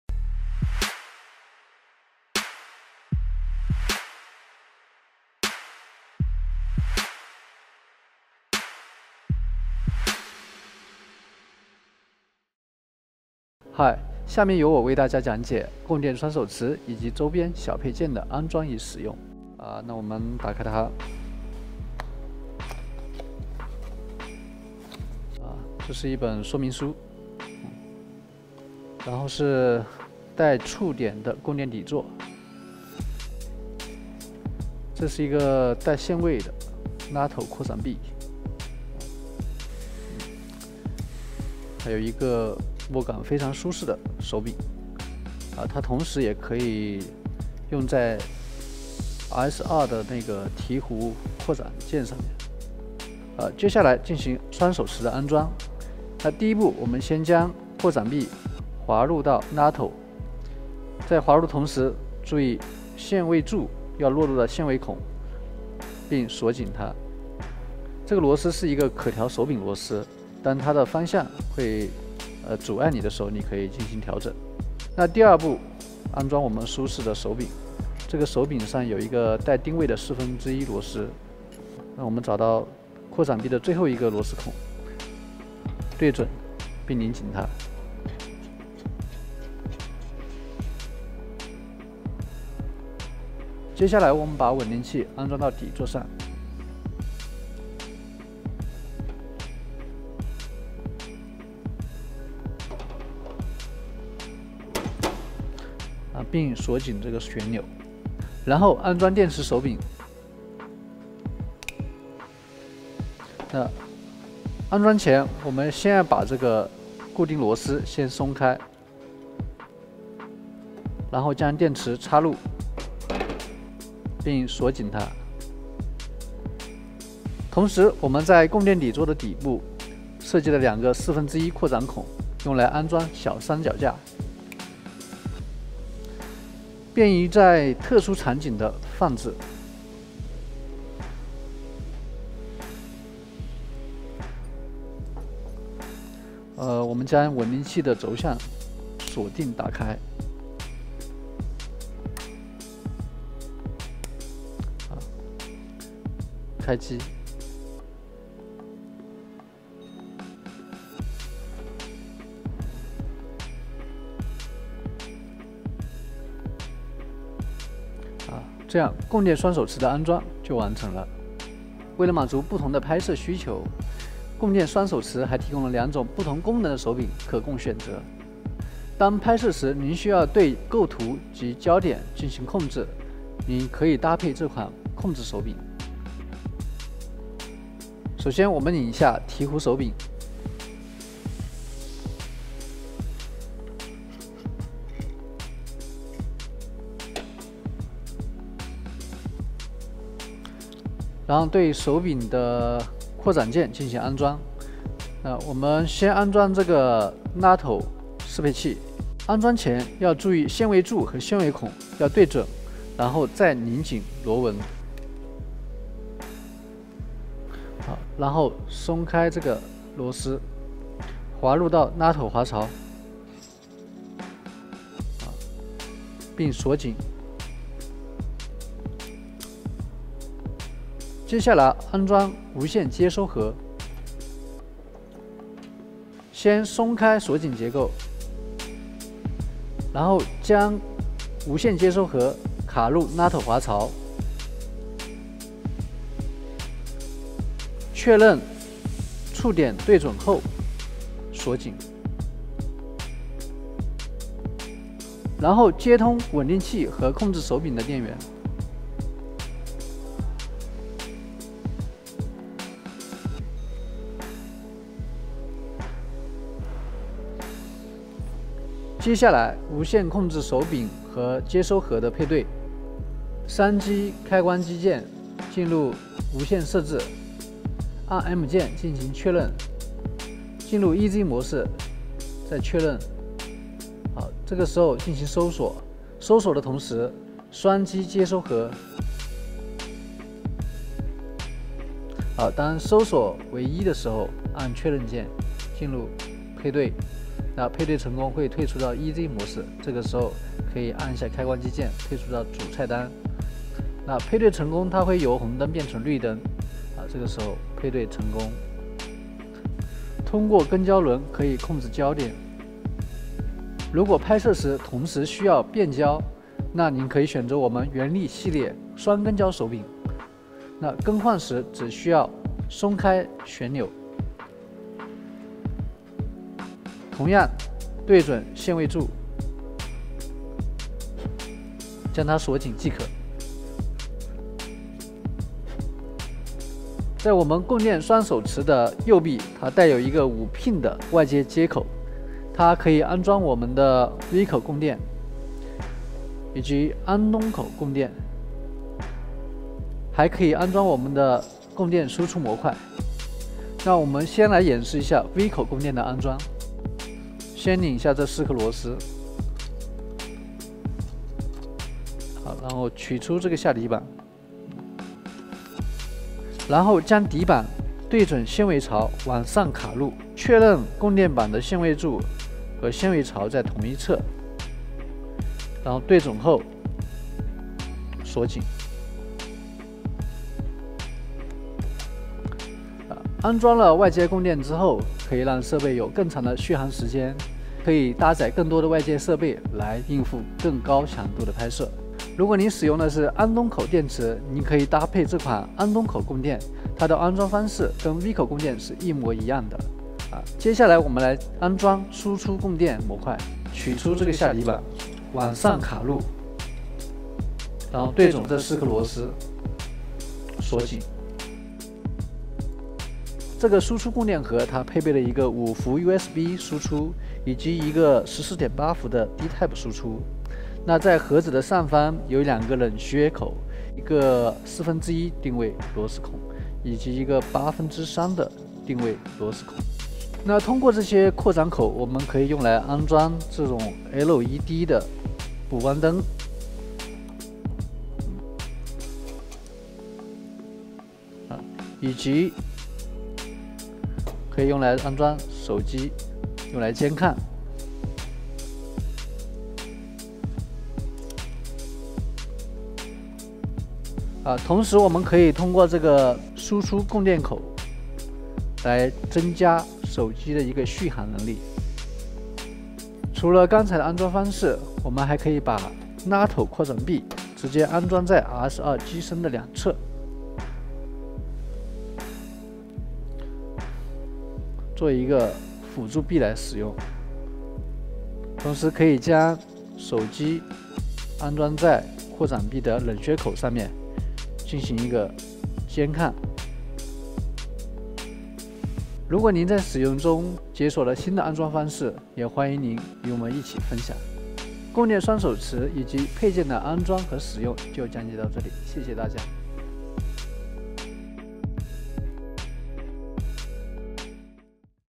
Tilta Dual Handle Power Supply Bracket for RS 2 Tutorial
Learn how to assemble and operate the Tilta Dual Handle Power Supply Bracket for RS 2.

The Dual Handle Power Supply Bracket for RS 2 allows you to power your RS2 via a Gold or V-Mount battery, provides additional stability when operating, and features multiple mounting points for attaching accessories.

The Bracket allows you to mount the RS2’s Battery Handle on the left side for additional battery life, while the threads on the right side allow for mounting a mechanical handle in various positions and distances from the gimbal body.

Timecode：
0:00 - Intro
0:19 - Unboxing
0:53 - Dual Handle Power Supply Bracket Assembly
1:37 - Install onto the Gimbal
2:52 - Optional Handles
3:11 - Installing the Wireless Lens Control Handle
3:53 - Installing the Wireless Control Receiver Module
4:30 - Pairing the Handle and Receiver Module
5:17 - Installing the Dual Channel Wireless Lens Control Handle
5:37 - 5-Pin External Power Supply Port Overview
5:55 - Installing V-Mount Battery Plate
6:32 - Gold Mount Battery Plate Overview
6:41 - Installing the Power Supply Module
6:53 - Using the Power Supply Module
7:43 - Additional Mounting Positions
8:10 - Closing